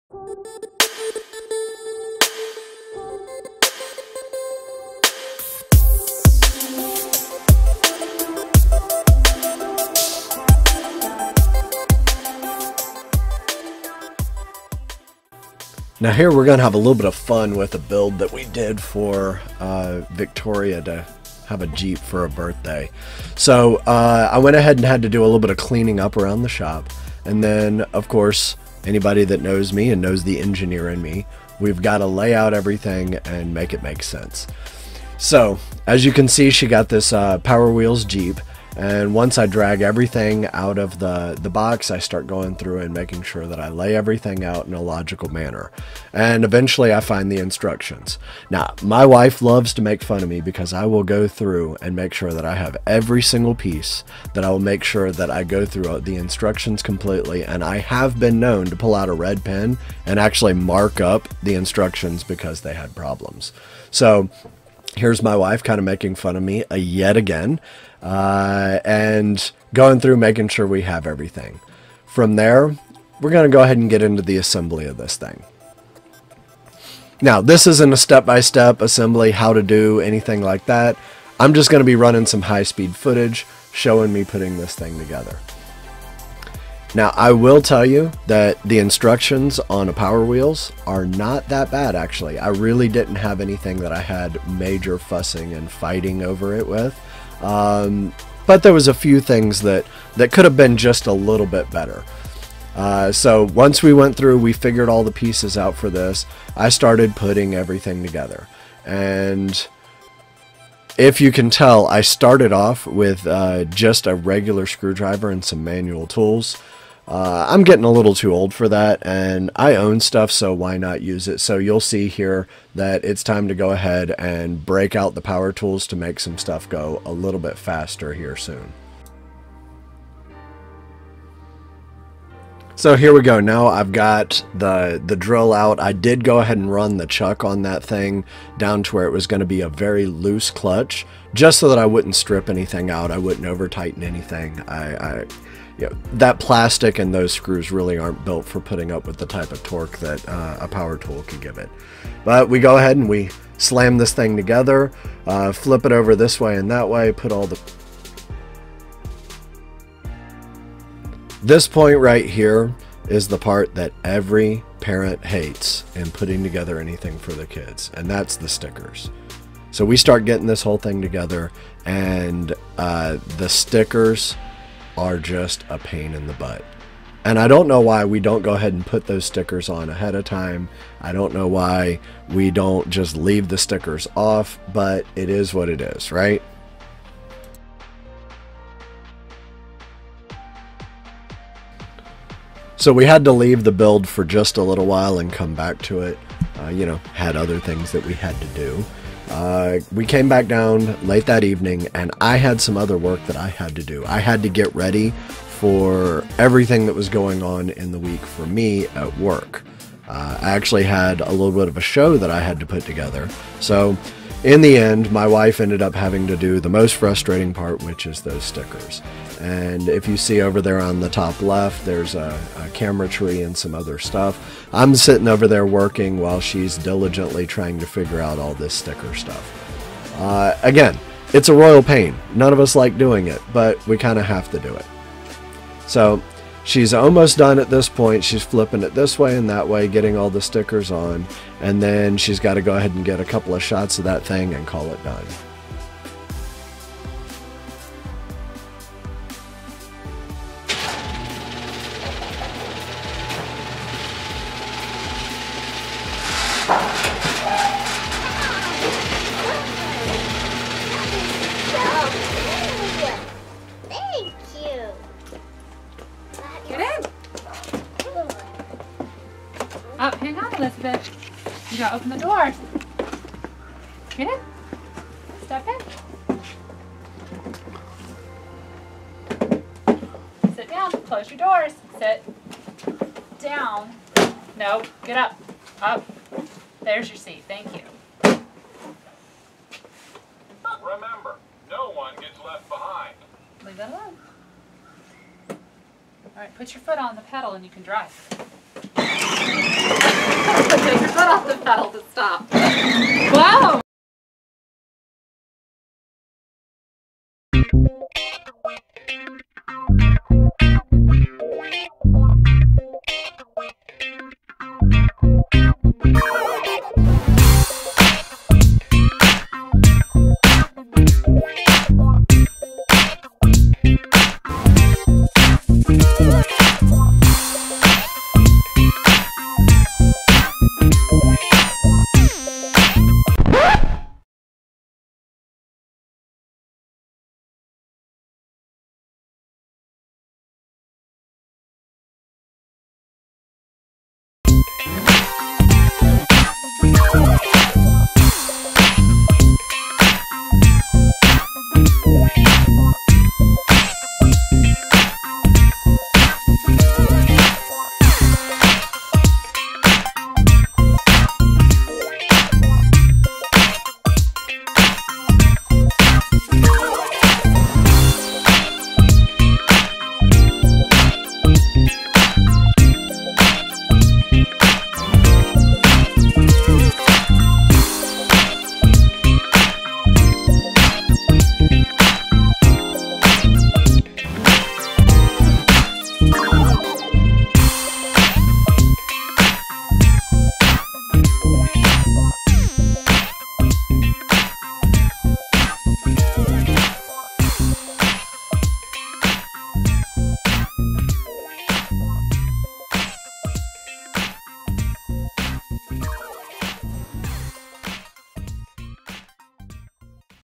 Powerwheels Jeep Build - Victoria's 4th Birthday.
Stephi and I build a Powerwheels Jeep for Victoria.  Follow it from box to the first drive in a quick time lapse set of shots with voiceover.

I also use my new Mic for the voiceover for the first time.  I need to work on setting the levels some still, but it is a massive improvement.